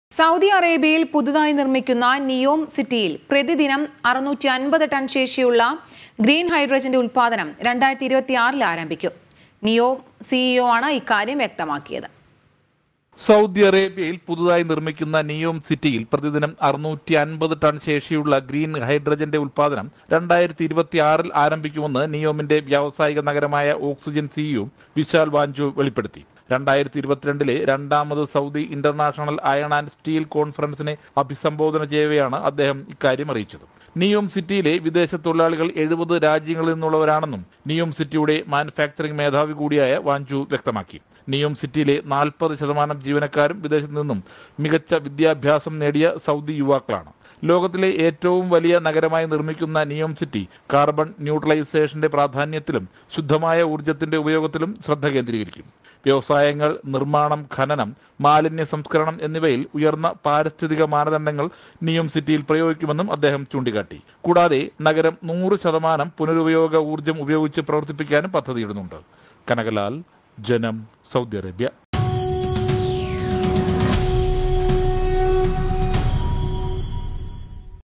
നിയോം സിറ്റിയിൽ പ്രതിദിനം 650 ടൺ ശേഷിയുള്ള ഗ്രീൻ ഹൈഡ്രജന്‍റെ ഉൽപാദനം ആരംഭിക്കും.
NEWS ANCHOR :  KEERTHANA